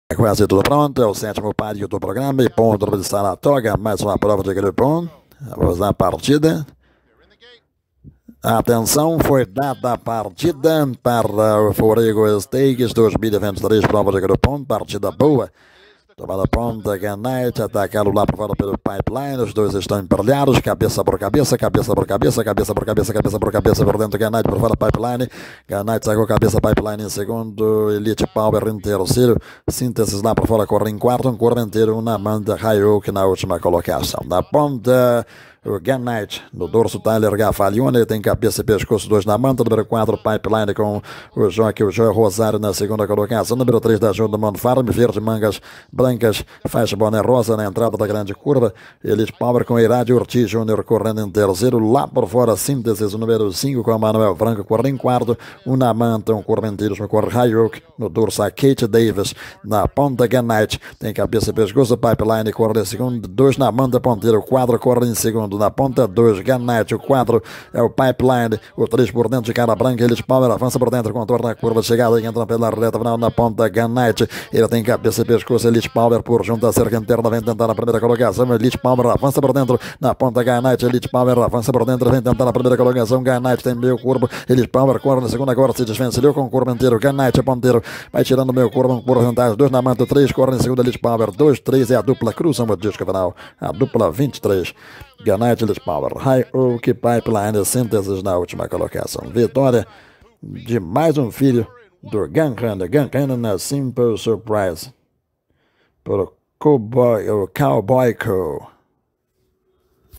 Forego Stakes (G1) 2023
Track: Saratoga Race Course (USA)
Winner: Gunite (Gun Runner - Simples Surprise - Cowboy Cal)